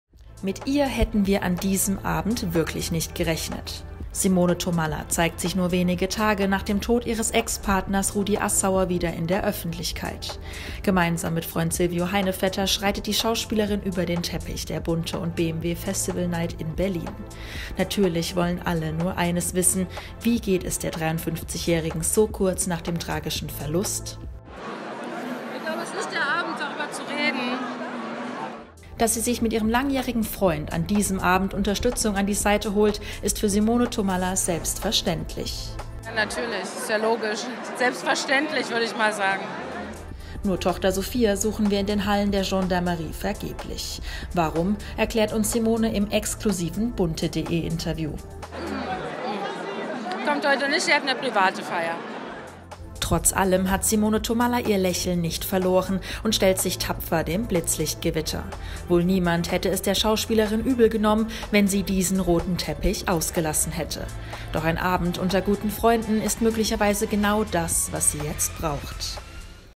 Simone Thomalla - Erster Auftritt nach dem tragischen Verlust ihres Ex-Partners Rudi Assauer (†74)
Simone Thomalla